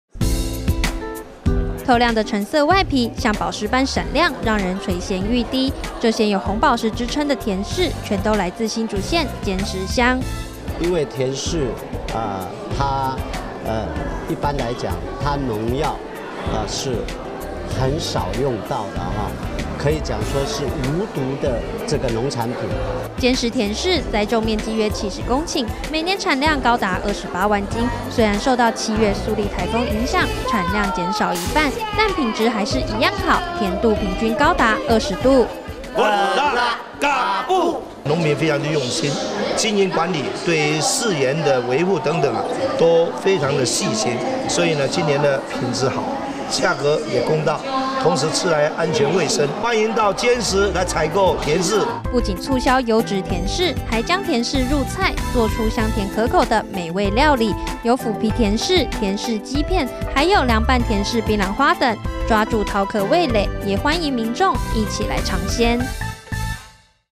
尖石鄉甜柿促銷記者會
新竹縣尖石鄉的甜柿果實肥碩，甜度高達二十度，邀請民眾來嘗鮮採購,體驗泰雅文化。